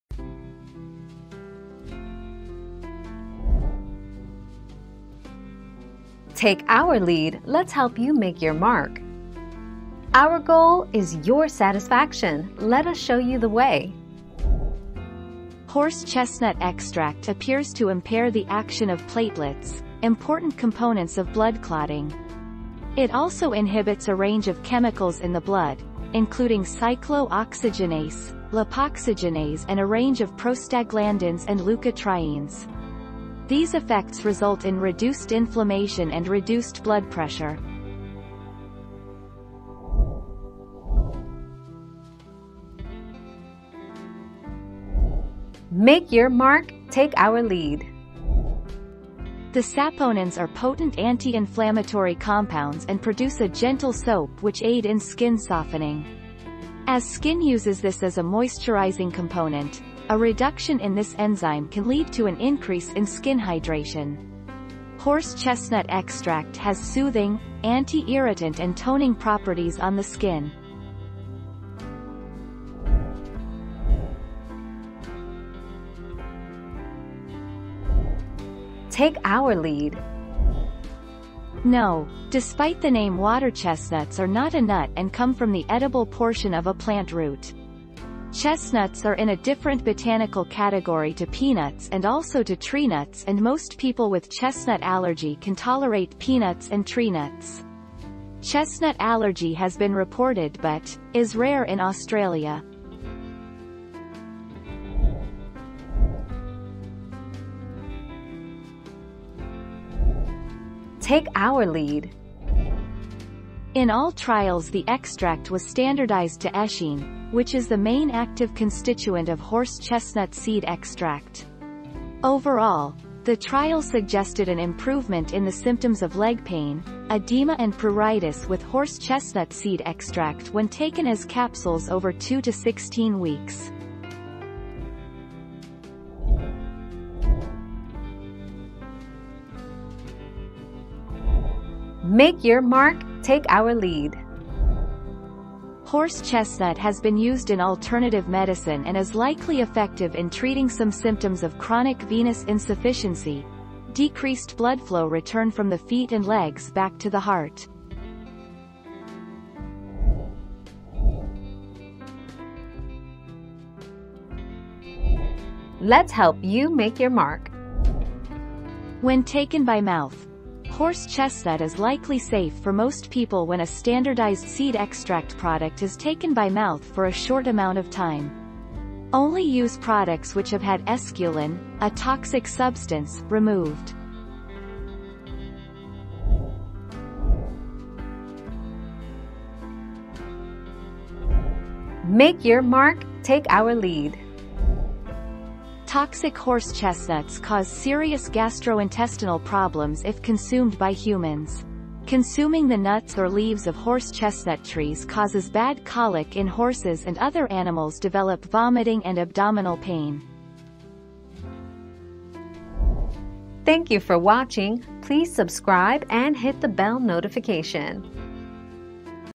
Does horse chestnut affect blood pressure?
00:00 - Does horse chestnut affect blood pressure?
00:41 - Is Horse Chestnut good for skin?
01:14 - Is horse chestnut a nut nut allergies?
01:46 - How long does it take for horse chestnuts to work?
02:17 - Is Horse Chestnut good for heart?
02:41 - Is it safe to take horse chestnut?
03:07 - What happens if you eat horse chestnuts?

Laura S. Harris (2021, June 8.) Does horse chestnut affect blood pressure?
    AskAbout.video/articles/Does-horse-chestnut-affect-blood-pressure-251803